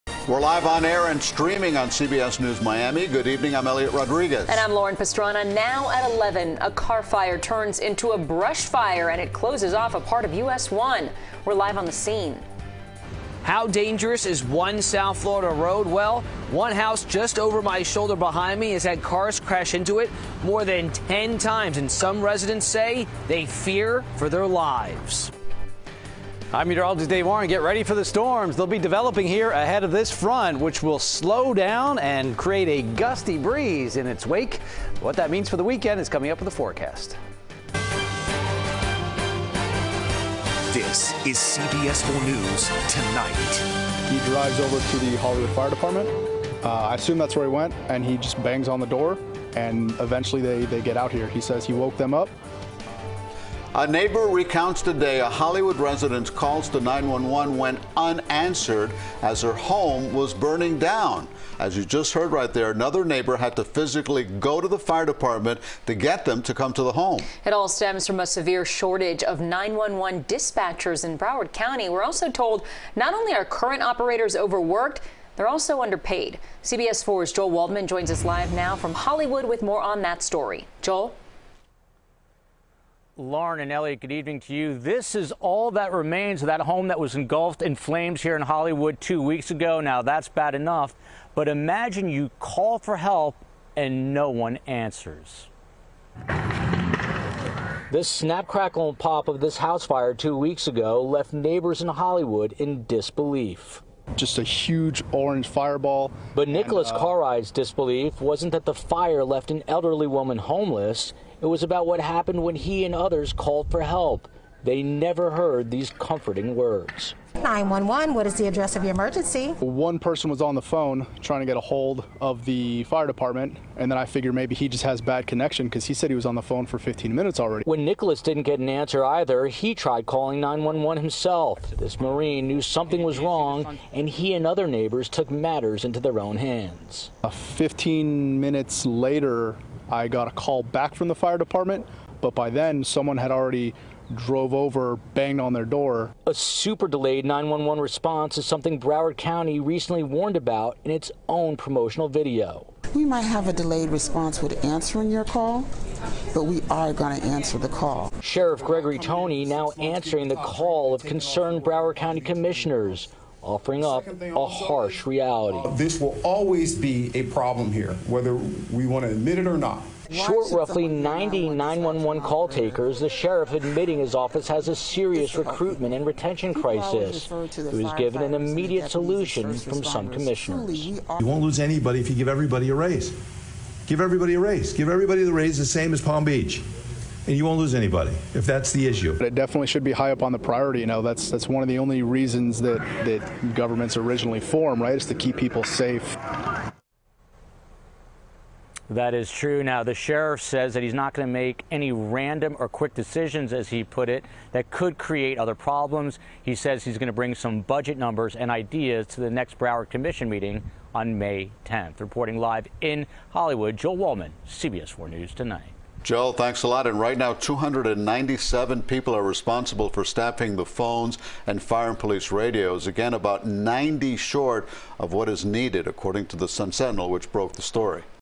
Broward County Suffering Critical 911 Dispatcher Shortage
A nationwide shortage of emergency 911 operators includes at least one South Florida county. Broward County is looking to fill close to 90 empty dispatcher positions.